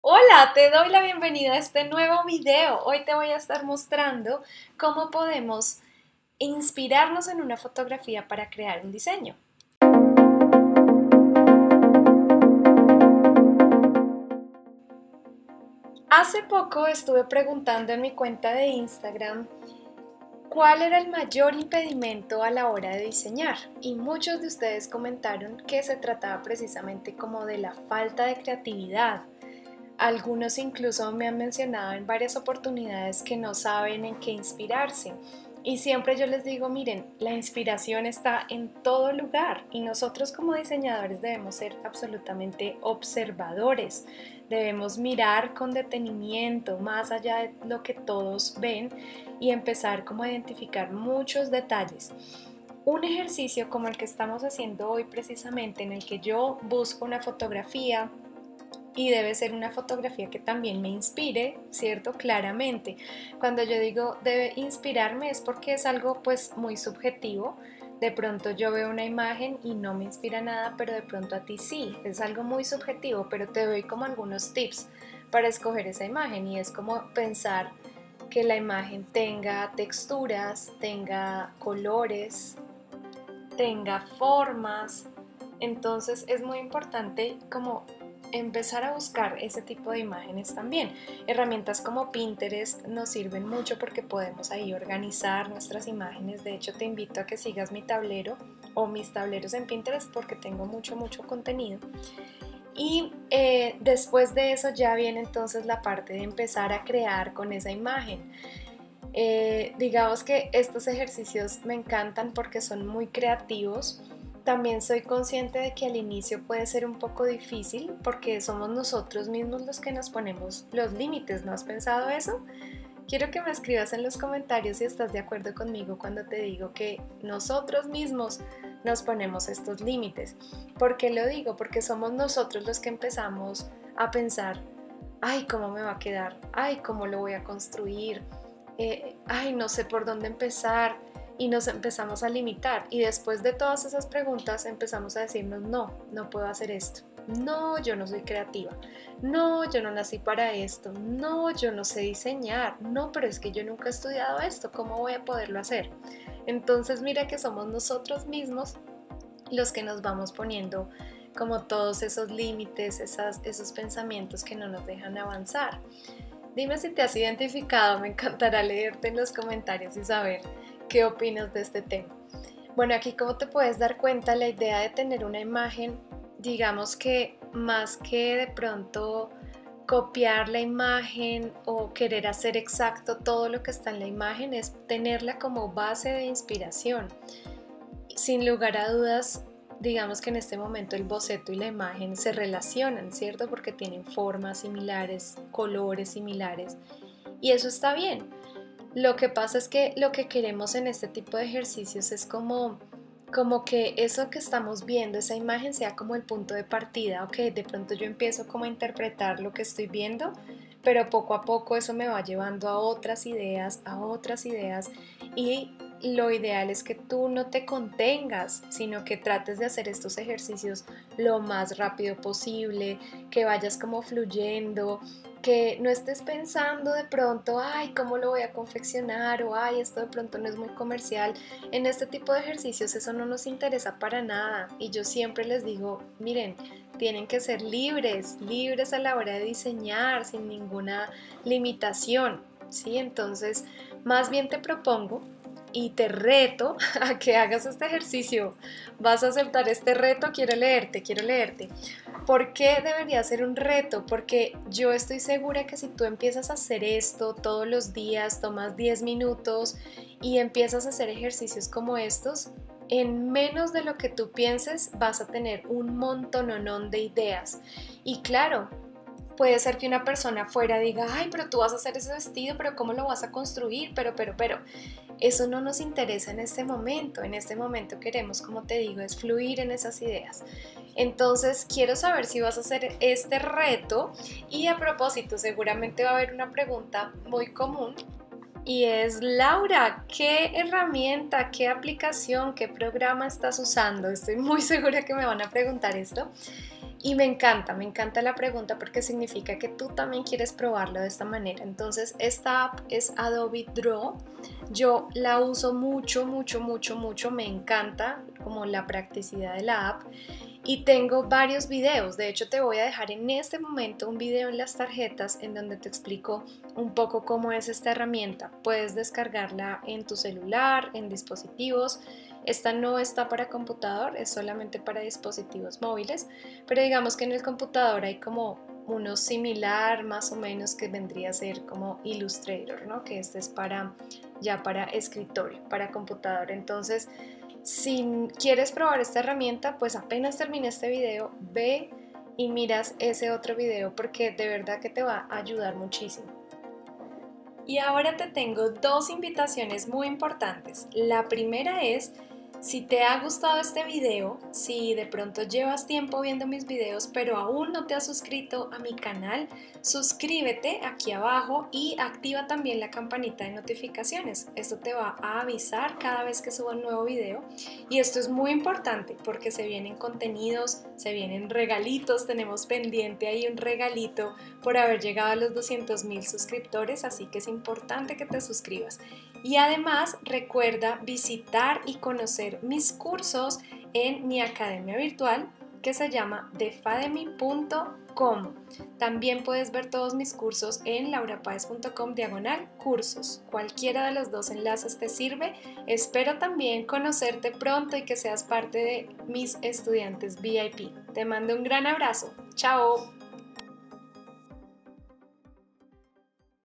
Cómo inspirarte para diseñar moda: Ejercicio con foto
Hoy tenemos un ejercicio creativo en el que vas a estimular tu creatividad y te vas a inspirar en una foto. Puedes ver mi proceso y usar esta misma foto o puedes buscar una foto que te inspire a ti.

Hablaremos de cómo los diseñadores debemos ser unos grandes observadores y que todo lo que hay a nuestro alrededor puede inspirarnos.

Puedes hacer este ejercicio de forma manual con lápices o marcadores, o también en la app que estoy usando que es AdobeDraw para tableta o celular.

Vamos a hacer juntos un boceto de diseño creativo mientras charlamos de cómo inspirarnos para diseñar. ¿Empezamos?

Foto por Matt Richardson en Unsplash